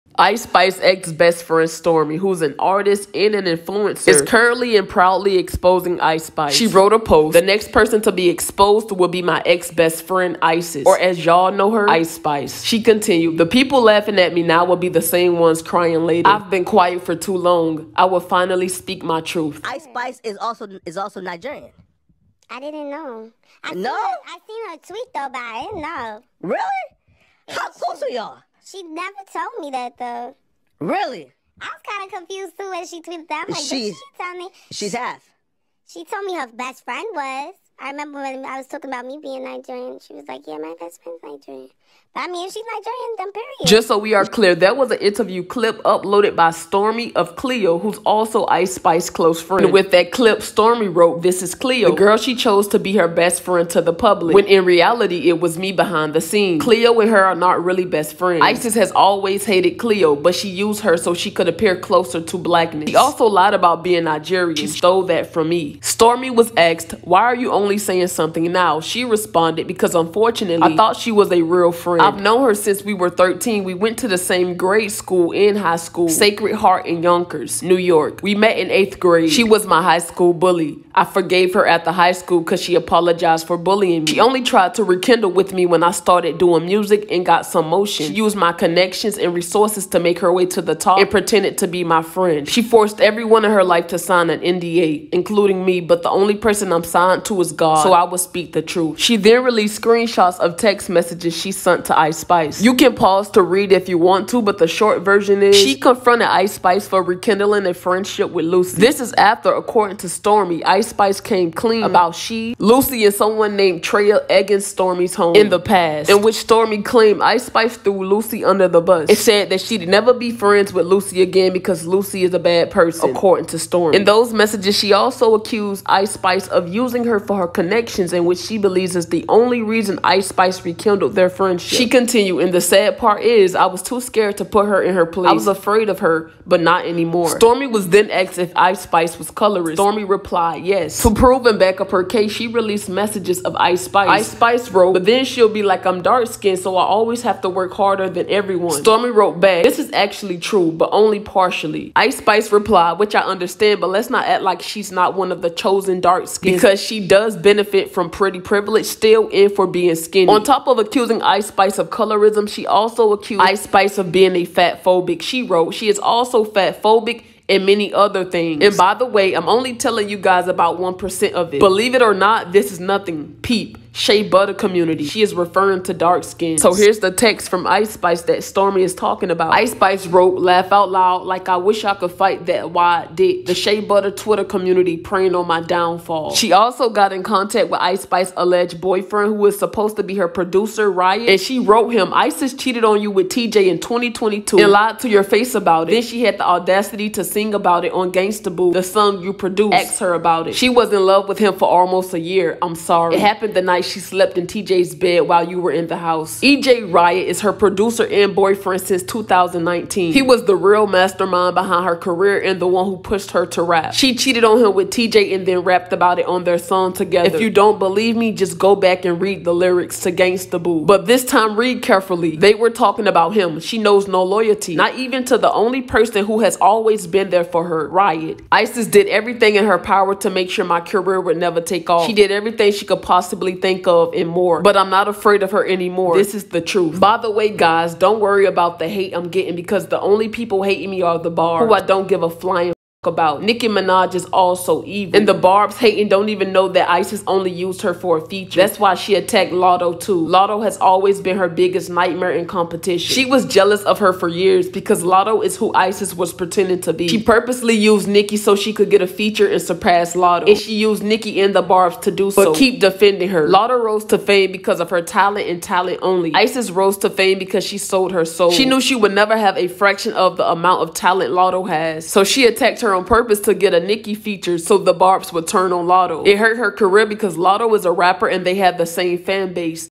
ICE SPICE EXPOSED BY HER EX BFF FOR CHEATING ON HER BOYFRIEND WITH LIL TJAY!
IceSpice ex bestfriend Stormee accuses her of cheating on her boyfriend with the rapper